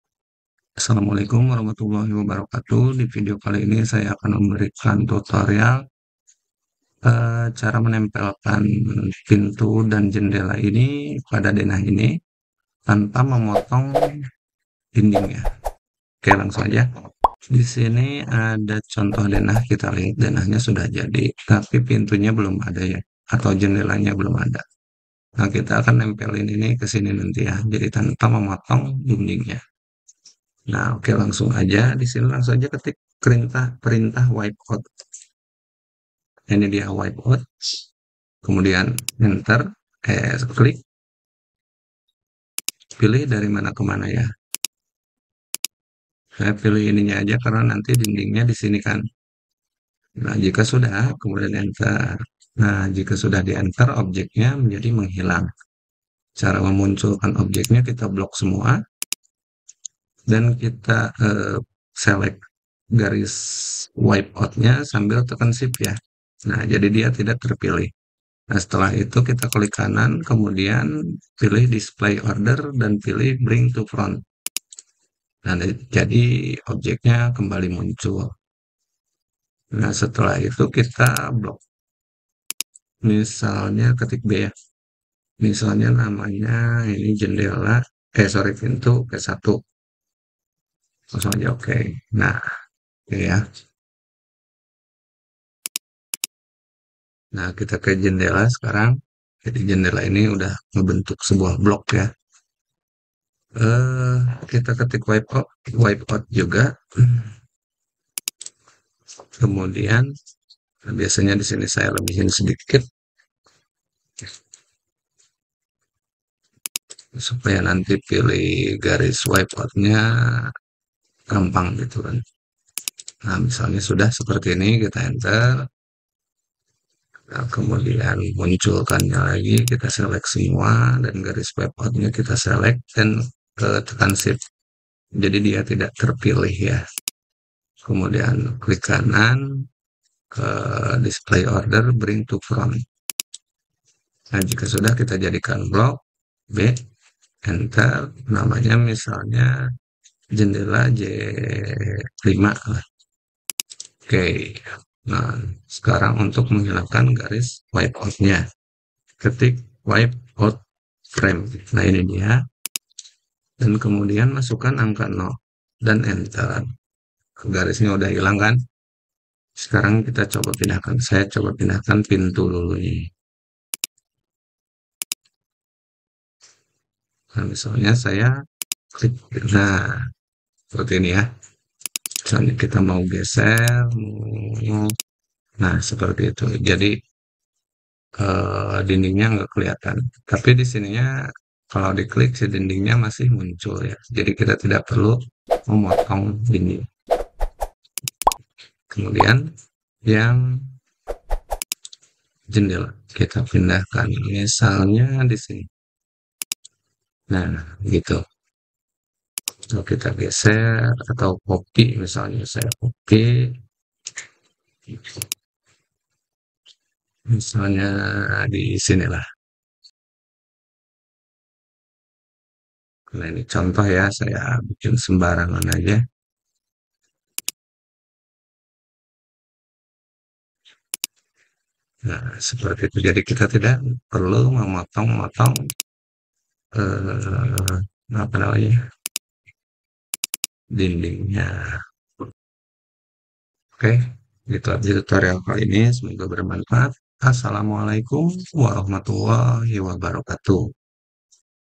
Menempelkan Jendela atau Pintu pada Denah Tanpa Memotong Dinding
Perintah Wipeout di AutoCAD berfungsi untuk menutupi area gambar dengan warna latar belakang saat ini. Perintah ini berguna untuk berbagai keperluan, seperti: Mengaburkan informasi sensitif, Menutupi detail yang tidak perlu, Meningkatkan tampilan gambar

Jual berbagai jenis software, bagi yang membutuhkan silahkan emailke